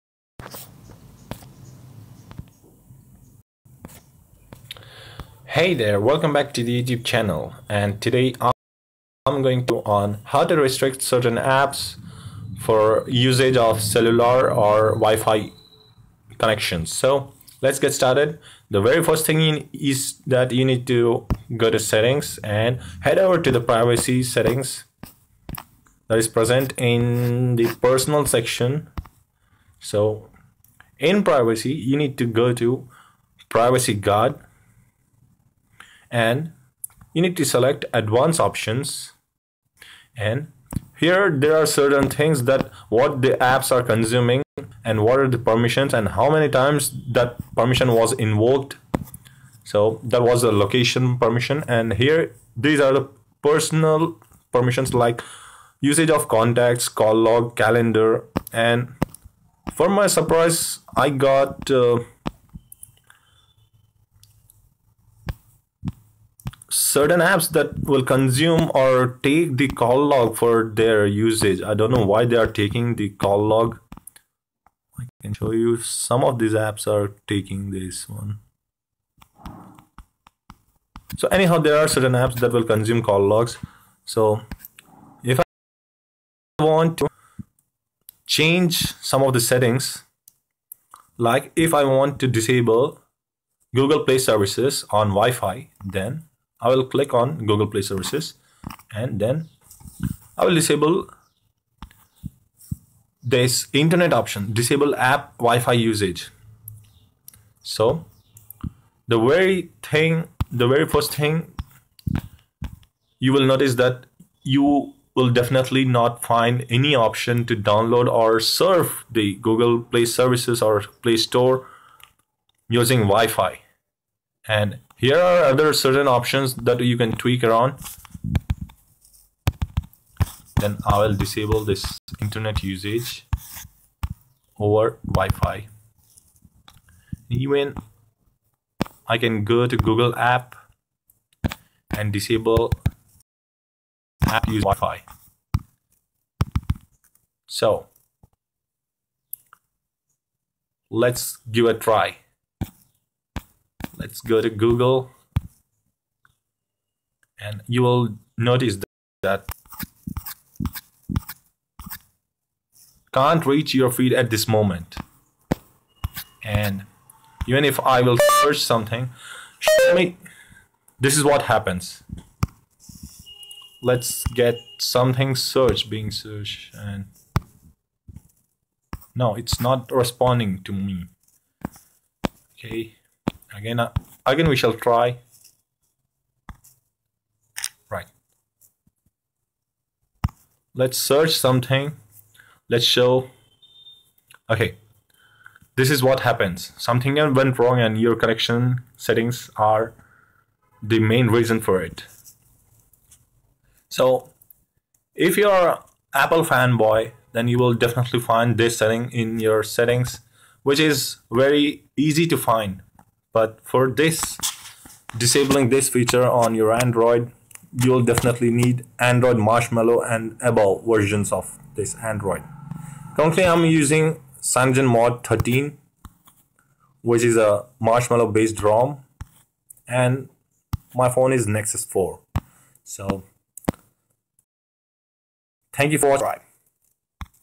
How to Restrict Internet access to certain Android apps on Android 6.0+
How to disable internet access to certain Android applications on Android 6.0 and Above.
In the tutorial I will show you how to restrict internet permission on a few apps with a demonstration

If the tutorial was helpful and you like the content that I'm sharing, then please contribute to the channel by hitting the like button, commenting your opinions and sharing them among friends.